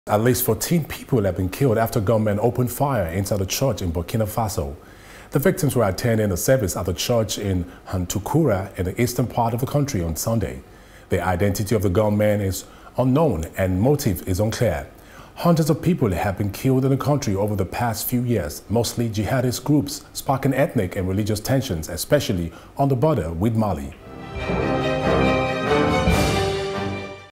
Burkina Faso: Attack On Church Kills At Least 14
At least 14 people have been killed after gunmen opened fire inside a church in Burkina Faso.

The victims were attending a service at a church in Hantoukoura, in the eastern part of the country on Sunday.

The identity of the gunmen is not known and the motive is unclear.